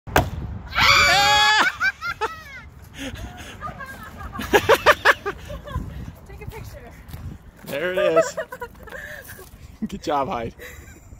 Heidi sticks the axe ... finally!
The Highland Axe Throwing Company, Ouray, Colorado